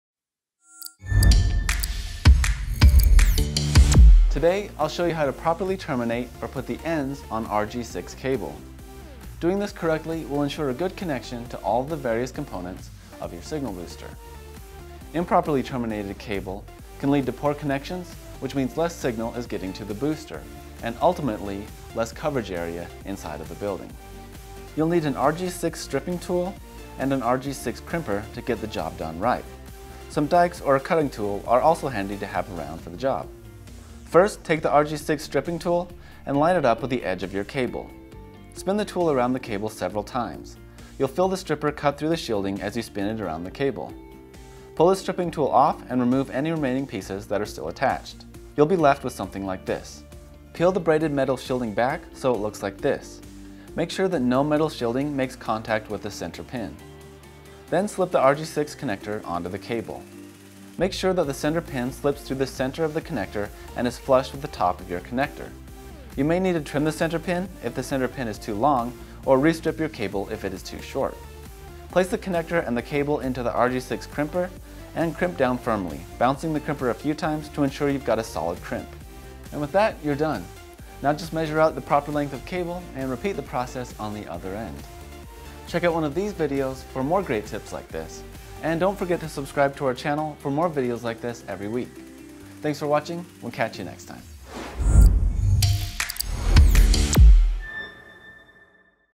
How to put the ends or connectors on RG-6 cable | weBoost
RG-6 is a common cable used in setting up a cellular signal booster as well as many A/V components. In this video we'll show you how to properly install the connectors or terminate the cable so you'll get the maximum amount of performance out of your cellular system.

Knowing how to properly terminate RG 6 cable will allow you to create custom cables for each specific run in your install and maximize the amplifier performance.

Poorly terminated cables can result in a bad connection and thus less signal getting through to the amplifier. The end result is less coverage area inside the building.

If you have more questions about WilsonPro check out our support center